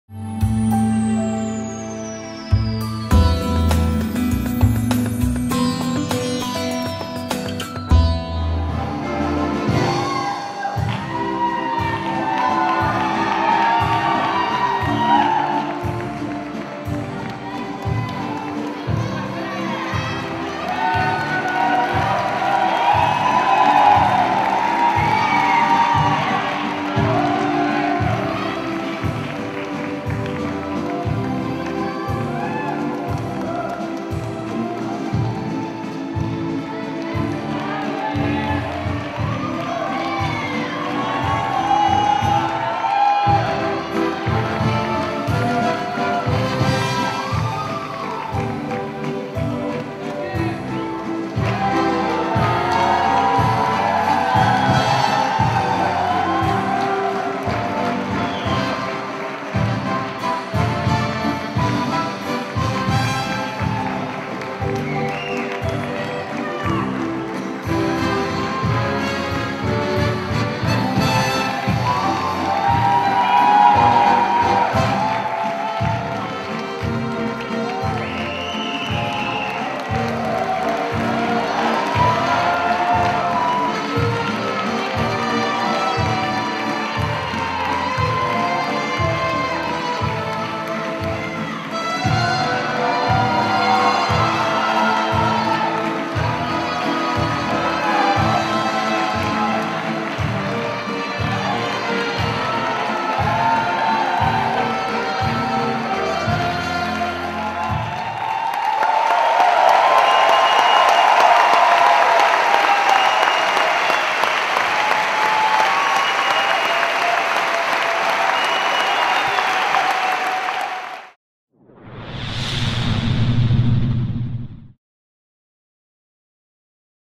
The Final Viennese Waltz | 2013 WDSF PD Super GP STD
Couples dance the Viennese Waltz in the Final of the 2013 WDSF PD Super Grand Prix Standard in Stuttgart, Germany on 13th August.